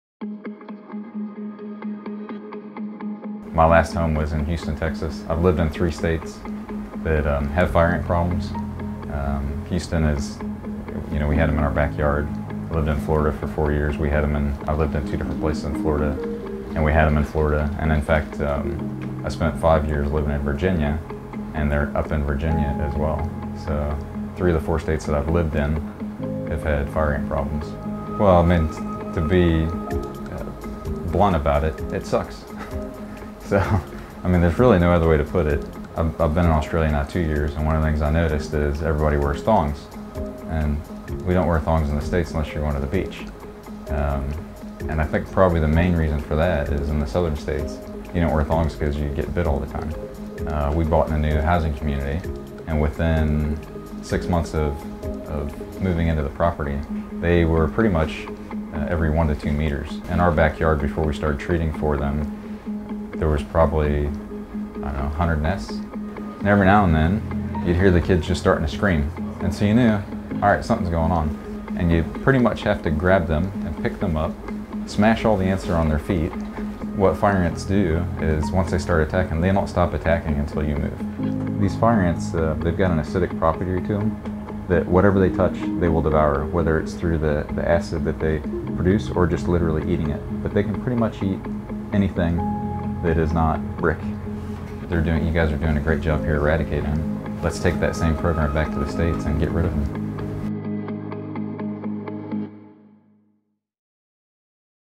Fire ant impacts in the United States
Patrick has witnessed the problem fire ants have caused in the United States. Join the ant hunt and help eradicate them once and for all.